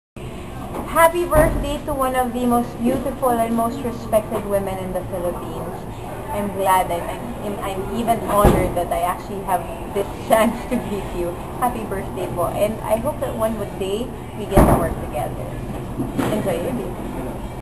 70 Celebrity Greetings for the 70th Birthday of the Queen, Ms. Susan Roces
Iza Calzado